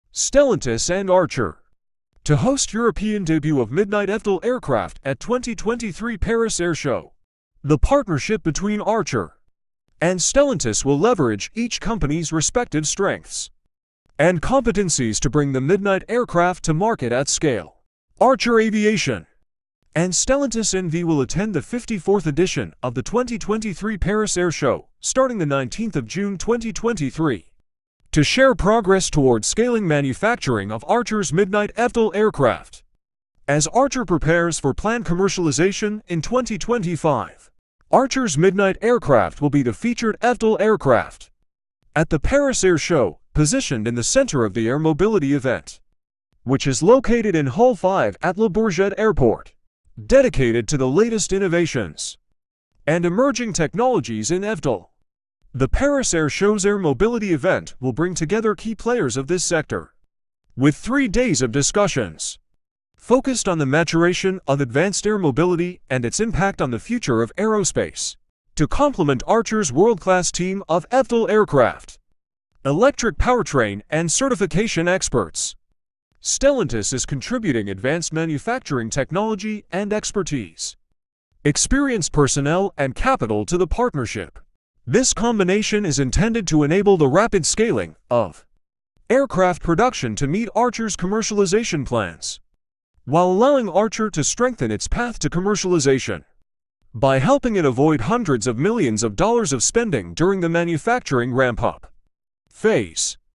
European debut of Midnight eVTOL aircraft at 2023 Paris Air Show
Stellantis and Archer have announced their plans to showcase the Midnight eVTOL aircraft for the first time in Europe at the 2023 Paris Air Show. This highly anticipated event, which commences on 19 June 2023, marks the 54th edition of the prestigious Paris Air Show. Archer Aviation and Stellantis N.V. will take this opportunity to demonstrate their advancements in scaling up the production of the Midnight eVTOL aircraft, as Archer gears up for its projected commercial launch in 2025.

At the Paris Air Show, the Midnight aircraft, a remarkable creation by Archer, will be prominently featured as the flagship eVTOL aircraft. Positioned at the heart of the Air Mobility event held at Hall 5 in Le Bourget Airport, this exhibition specifically focuses on presenting the cutting-edge innovations and emerging technologies within the eVTOL sector. Over the course of three days, industry leaders and key stakeholders will engage in discussions centered around propelling air mobility forward and exploring its profound impact on the future of aerospace.

The partnership between Archer and Stellantis is strategically founded on harnessing the unique strengths and competencies of each company. Archer excels in the development of eVTOL aircraft, electric powertrains, and certification processes, while Stellantis brings to the table advanced manufacturing technology, expertise, skilled personnel, and capital. This collaborative effort aims to expedite the scaling of aircraft production, facilitating Archer's realization of its commercialization goals. Furthermore, the partnership safeguards Archer from incurring significant expenses during the manufacturing ramp-up phase, potentially saving hundreds of millions of dollars.

Please note that the information provided is based on the text you provided and my training data up until September 2021.